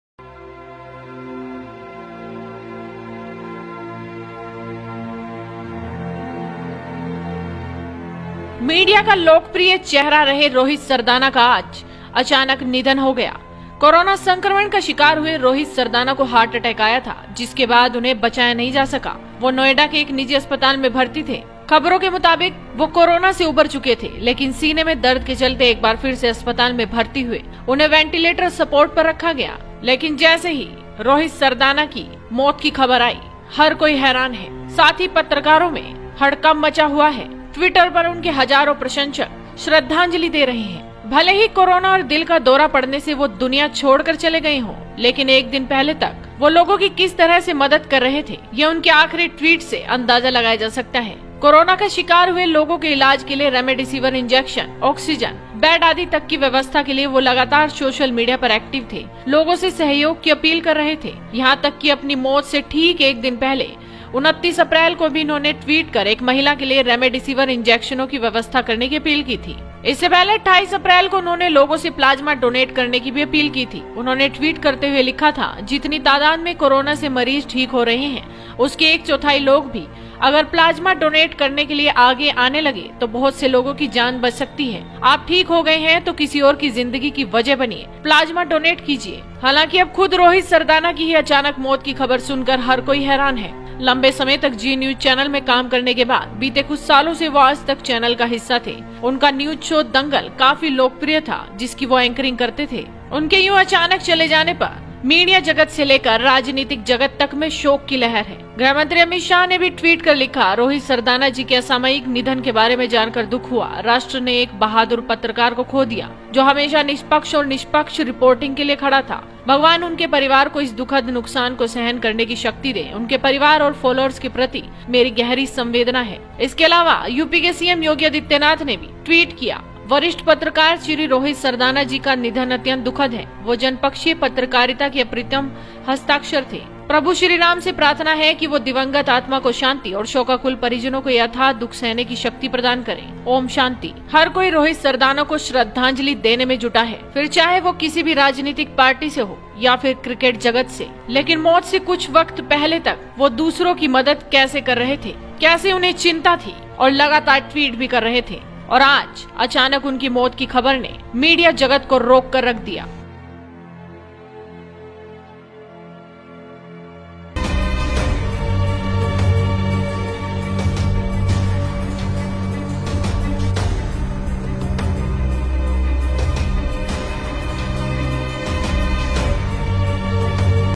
Rohit sardana का वो ट्वीट जिसकी पूरा देश कर रहा है तारीफ
जाने-माने टीवी पत्रकार रोहित सरदाना ने शुक्रवार को घातक COVID -19 संक्रमण से दम तोड़ दिया। ज़ी न्यूज़ के प्रधान संपादक सुधीर चौधरी ने एक ट्वीट के जरिए उनके असामयिक निधन की जानकारी देते हुए कहा कि 'इस खबर के लिए मैं बिल्कुल तैयार नहीं था।'